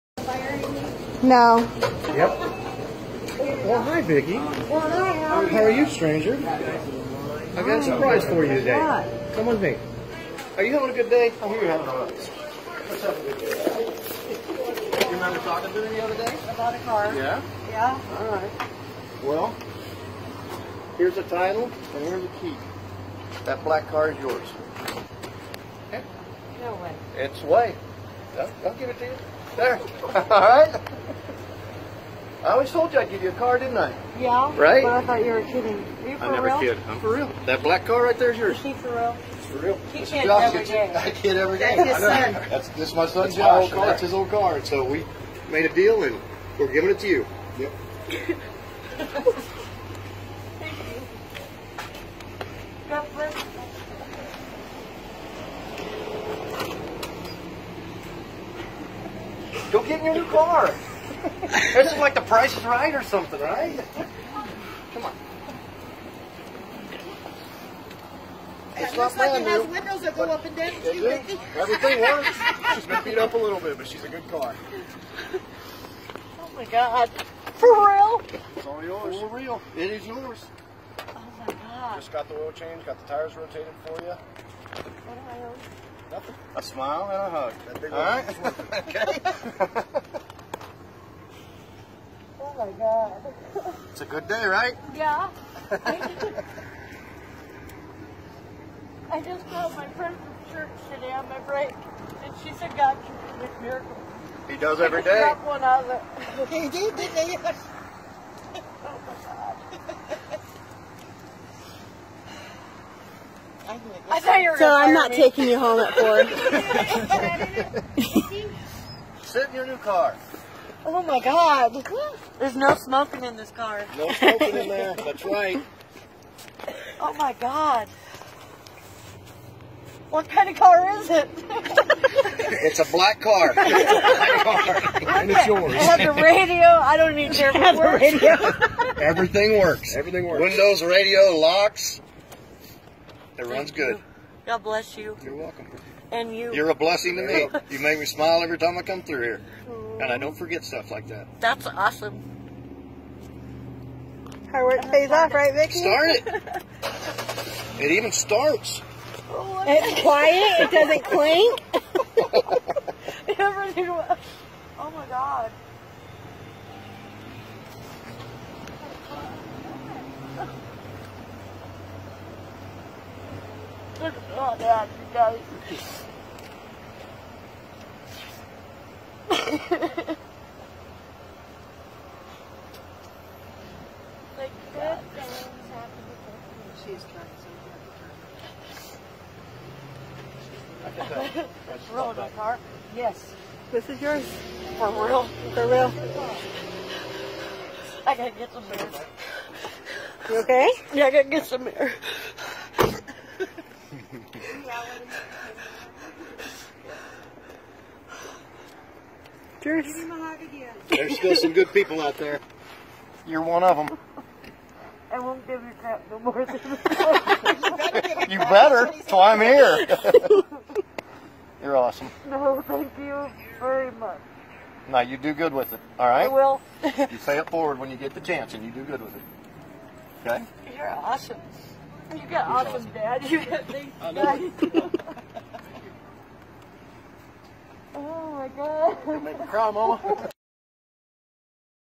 Customer Gives McDonald's Employee A Car
After discovering this McDonald's worker no longer had a way of getting to work, a kind-hearted customer stepped in with the most incredible gesture!